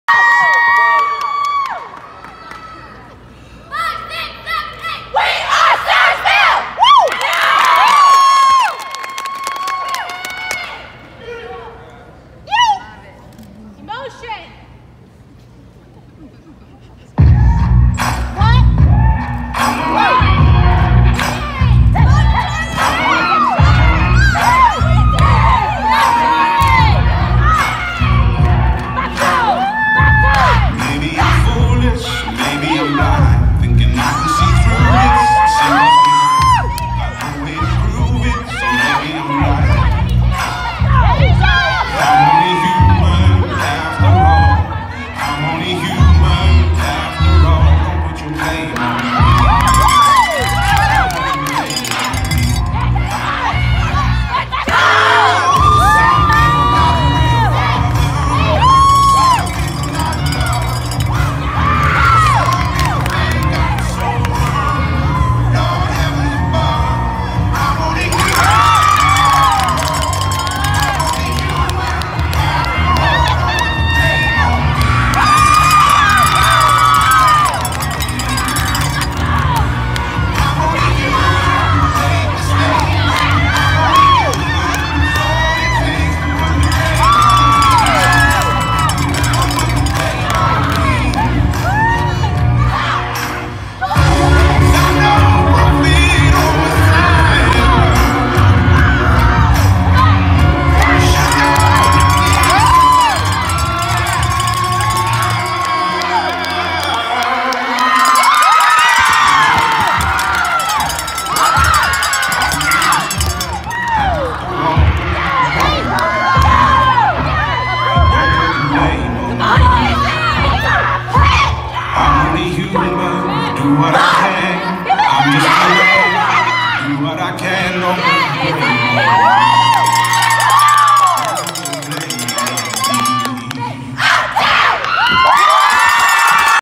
SMHS Pantherette Dance Team-GA jazz Champions 2019
choreography by Nichole Martin and coached by Sandi Johnson 2018-19 team. won 1 place - GA jazz state champions